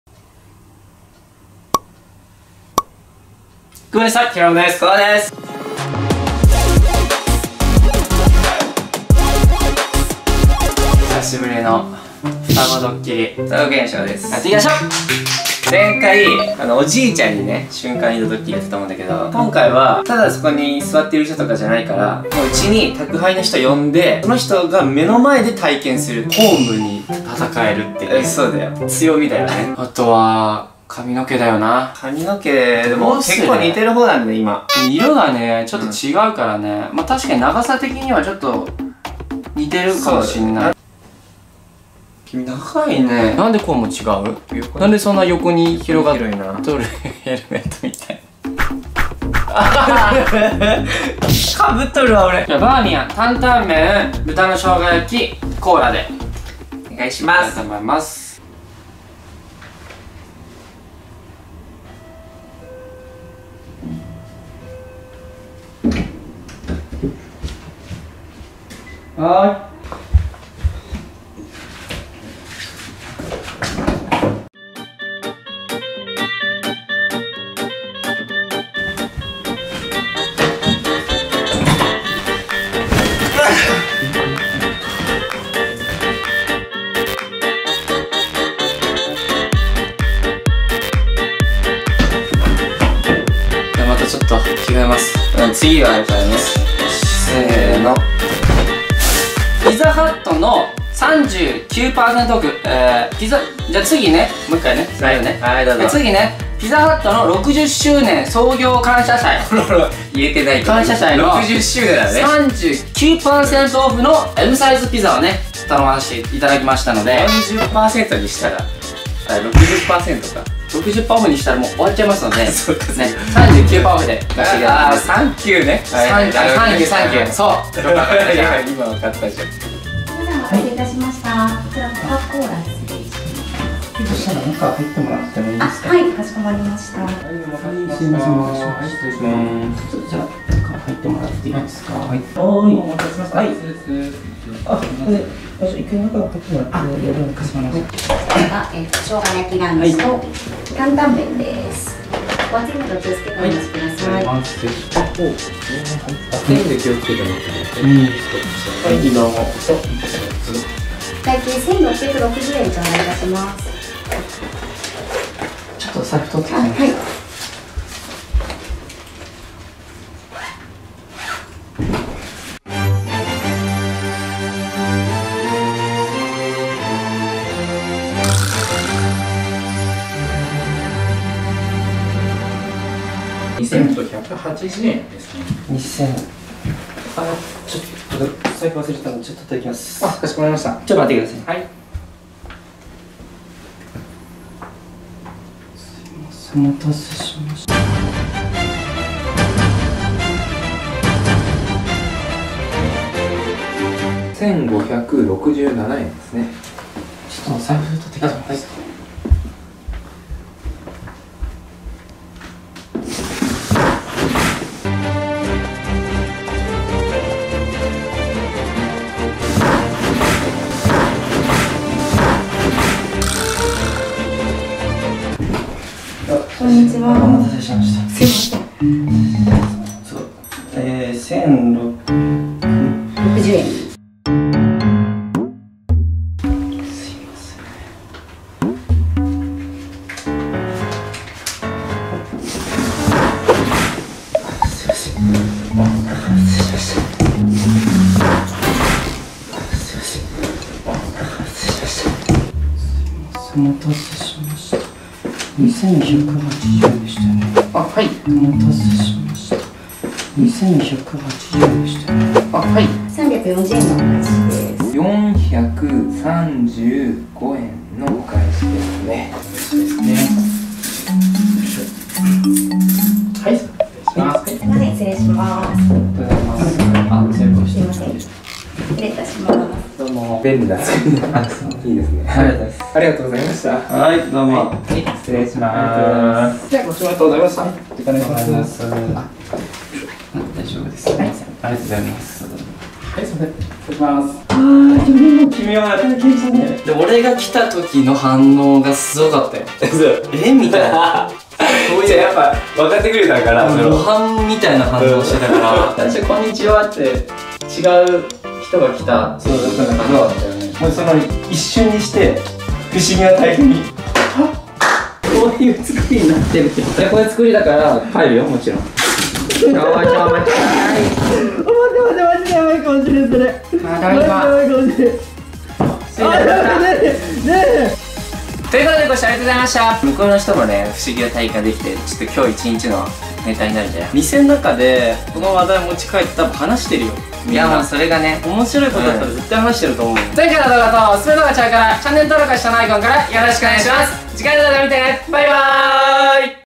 宅配員に双子が瞬間移動ドッキリしたら二度見が止まらなかったw
コウがピザ大好き人間なんです。でも毎週食べてるわけじゃありませんよ？
最近コウの舌が肥えてきてて高いピザが食べたいって言い出したらどうしようと不安です。
でも普通にデリバリーでピザ頼んだら高いので50％OFFの時にしか注文はしません、今回は39％OFFでしたが(汗
最近だと池袋に行った時に西口で入ったピザ屋が美味しかったので是非みなさんも行ってみてください！名前は忘れちゃいましたがあそこはお勧めです！
あとバーミヤンの担々麺が個人的に好きでした。

■愛猫のちゃちゃマロを見たい方はこちら

■TWINSのオフ感や撮影の裏側を見たい方はこちら